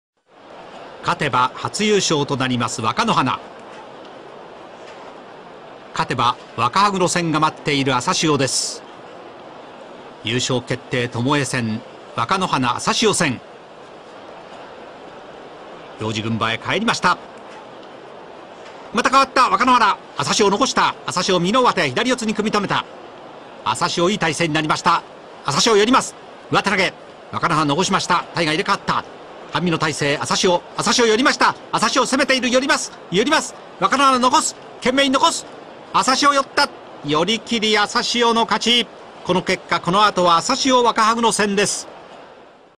Asashio vs. Wakanohana : Haru 1956 (朝汐 対 若ノ花)
Kettei-sen de l'Haru 1956
2e combat du Kettei-sen
Avec 12 victoires, l'Ozeki Wakanohana, le Sekiwake Asashio et le Maegashira Wakahaguro terminent le tournoi à égalité - un Kettei-sen à trois est organisé.
Après avoir remporté le 1er combat, Wakanohana affronte Asashio.
Si l'Ozeki l'emporte, il gagne le Yusho.
Victoire du Sekiwake Asashio par Yoritaoshi.
Asashio peut maintenant remporter la Coupe de l'Empereur, s'il gagne le combat suivant face à Wakahaguro.